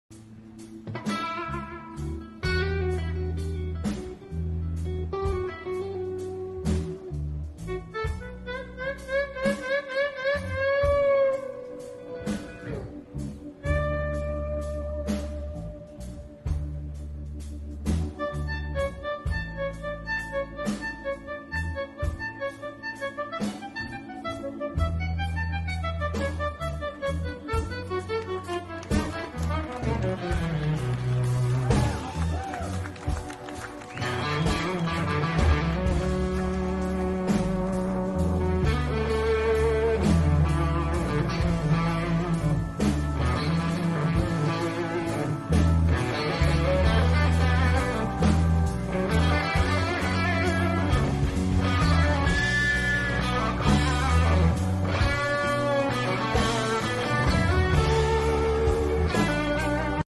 Skilled guitarist makes his guitar sing and almost duplicate the sound of a violin
Go Fun - Skilled guitarist makes his guitar sing and almost duplicate the sound of a violin